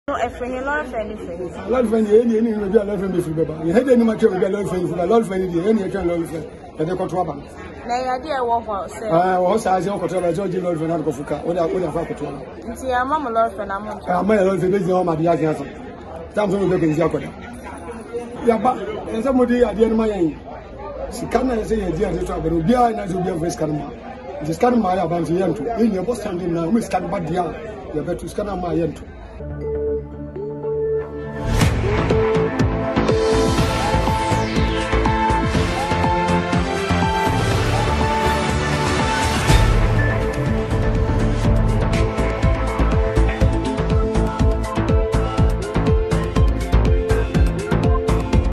NPP: Some delegates at the Asokwa constituency vow not to vote until they are given transportation
NPP Primary: Some delegates at the Asokwa constituency vow not to vote until they are given transportation.